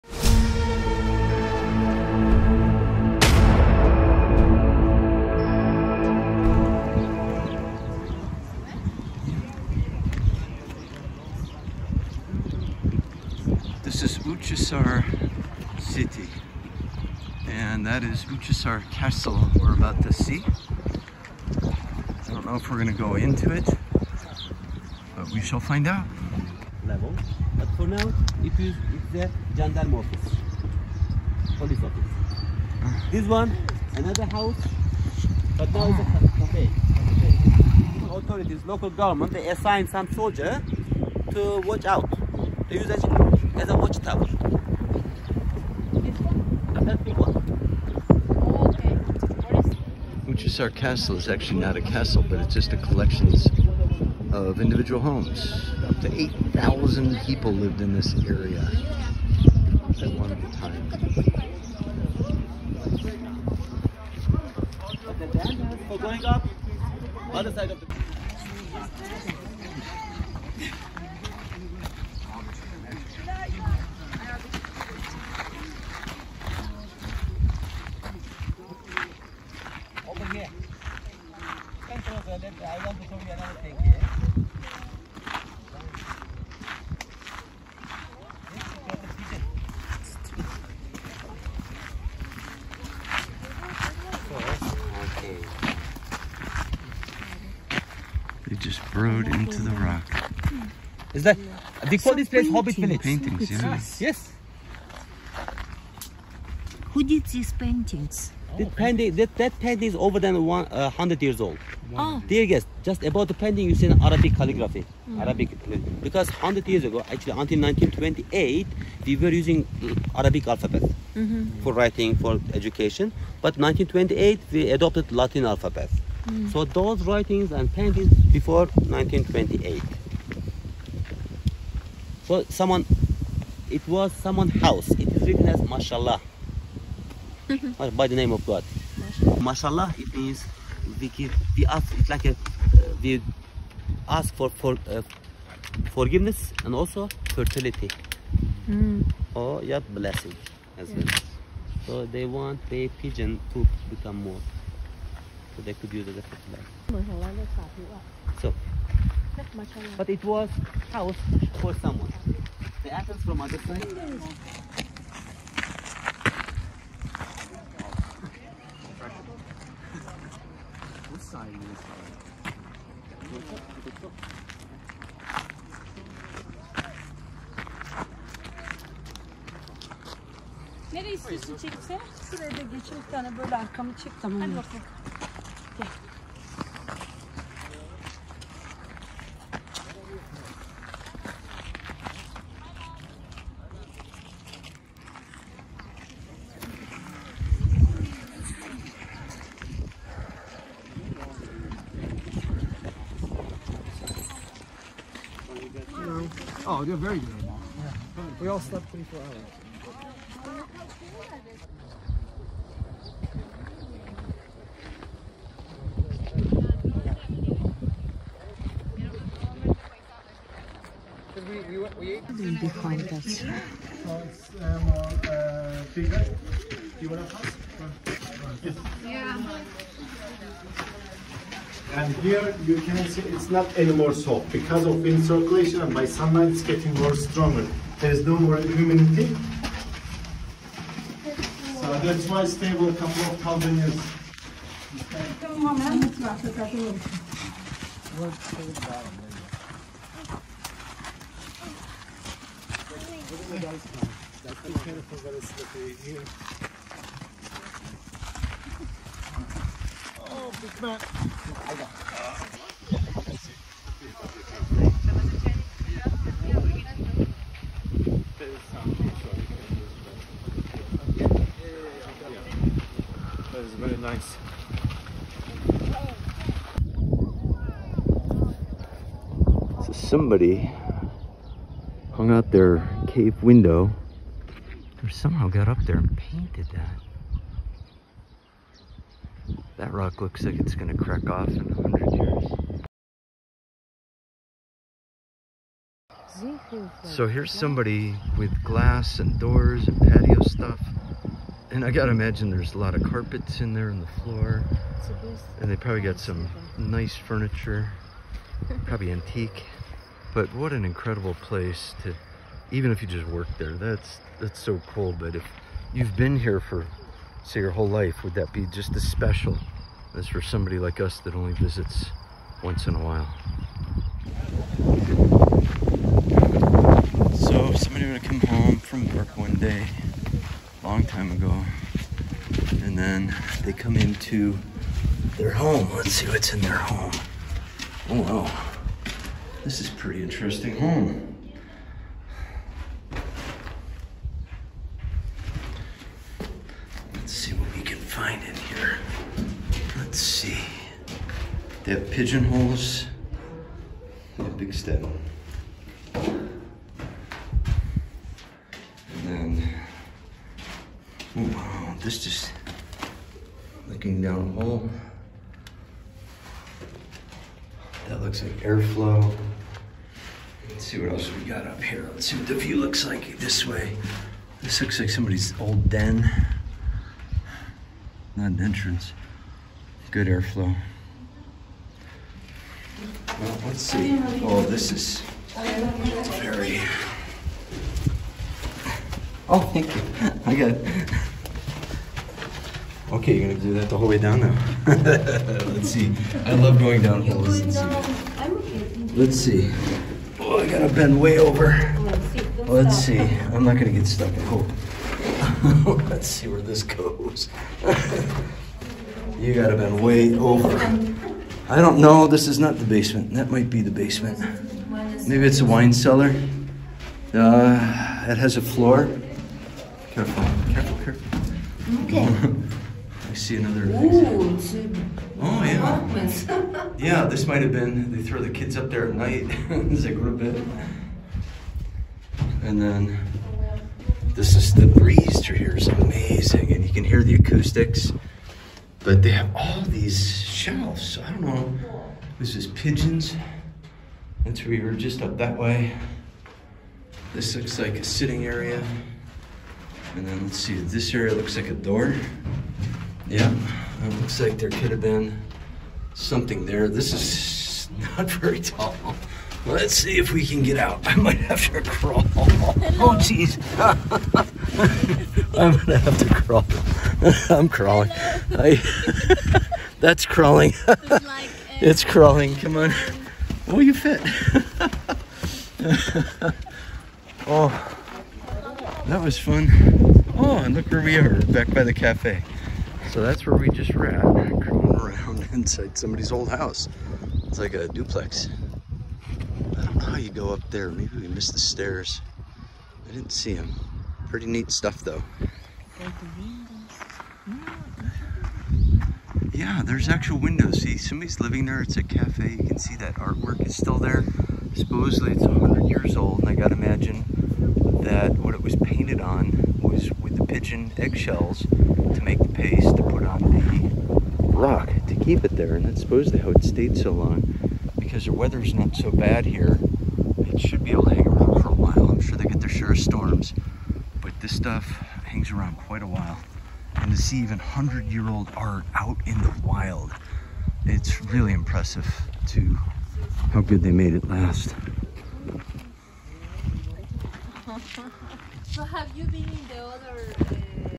Exploring Uchisar Castle: You Won't Believe What We Found!
Exploring Uchisar Castle: Part 4 - You Won't Believe What We Found! To see more of what the Red Tour has to offer, check out Part 1 to see where it starts. We greatly appreciate your support!

The aerial shots were recorded with the HoverAir X1

The Red Tour takes you into the Zelve open-air museum which is landscapes, caves, and canyons.
Love Valley, (Turkish Aşıklar Vadisi) is a valley in Göreme Historical National Park, Cappadocia, Turkey. It is known for its rock formations called fairy chimneys.

Zelve is an open valley with a large cave settlement. The small area has fifteen Byzantine-era cave churches and remained a Turkish village until 1960.

Cappadocia, Türkiye, is a mesmerizing region known for its unique geological formations, ancient history, and rich cultural heritage. This UNESCO World Heritage Site features surreal landscapes characterized by fairy chimneys, cave dwellings, and intricate rock-cut churches. Visitors can explore the underground cities of Derinkuyu and Kaymakli, hike through the stunning valleys, and experience the breathtaking landscape view from a hot air balloon ride. Cappadocia's captivating beauty and historical significance make it a must-visit destination for travelers seeking an unforgettable adventure.

Chapters
00:00 The Red Tour in Cappadocia Turkiye
00:30 Uchisar Castle
01:39 "Hobbit Village"
02:42 Uchisar City Landscape
04:37 How Much Longer Can It Last Before Collapsing?
04:57 A Modernized Cave House
05:27 Inside an Ancient Dwelling
07:55 Centuries Old Dwellings and Artwork